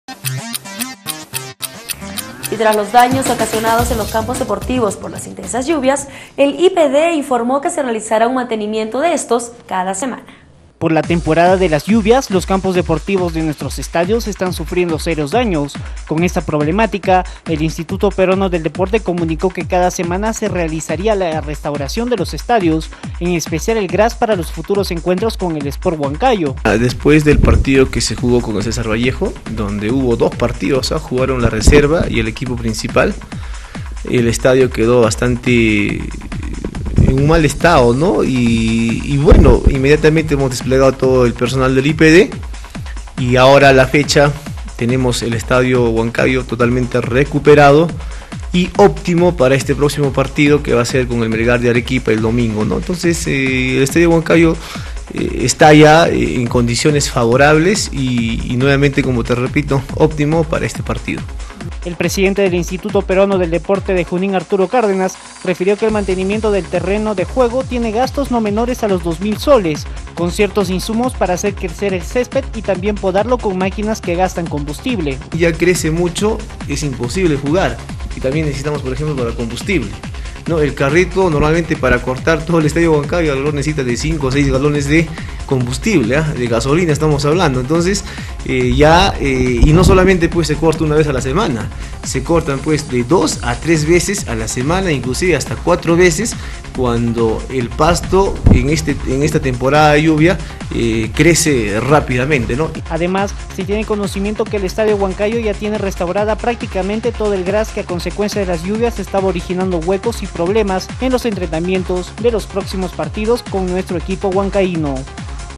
ASEGURAN QUE CAMPOS DEPORTIVOS ESTÁN BIEN.
Tras los daños ocasionados en los campos deportivos por las intensas lluvias, el Instituto Peruano del Deporte informo que se realizara el mantenimiento de estos cada semana